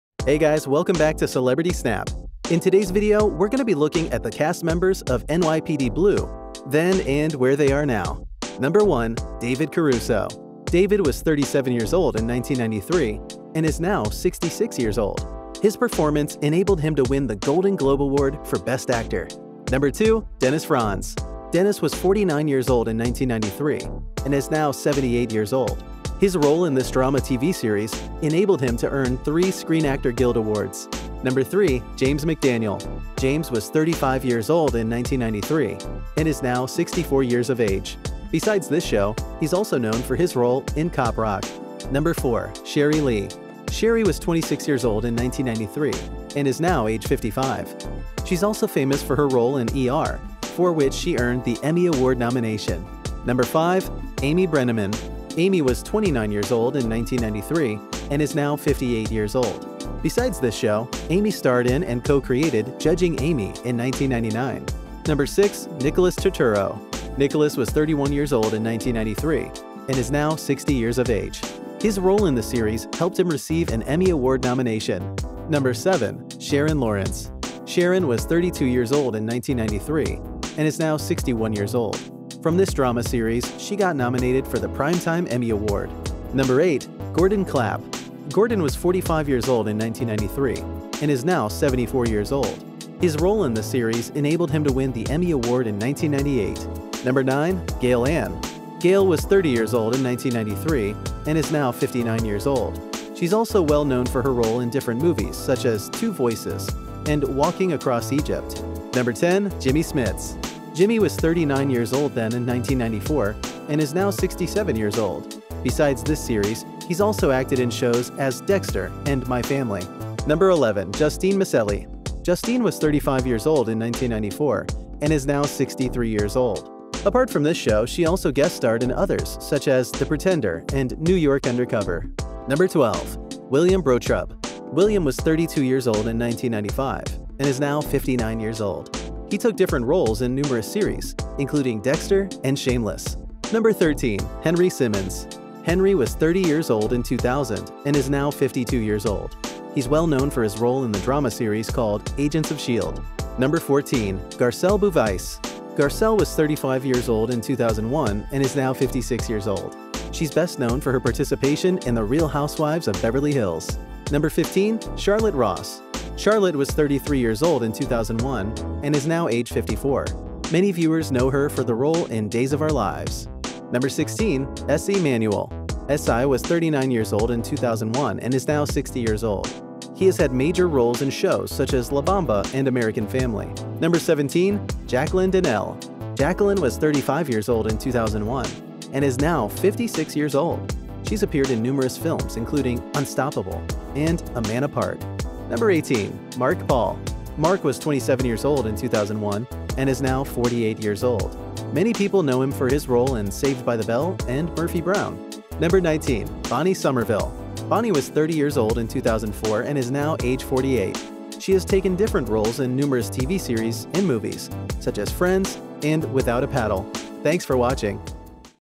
NYPD Blue Cast ✦ The Transformation | They have really grown old
NYPD Blue (1993) Cast ✦ The Transformation | They have really grown old
NYPD Blue (1993) Main Cast:
David Caruso
Dennis Franz
James McDaniel
Sherry Lea
Amy Brenneman
Nicholas Turturro
Sharon Lawrence
Gordon Clapp
Gail Ann
Jimmy Smits
Justine Miceli
William Brochtrup
Richard Bartlett
Henry Simmons
Garcelle Beauvais
Charlotte Ross
Esai Manuel
Jacqueline Danell
Mark-Paul
Bonnie Somerville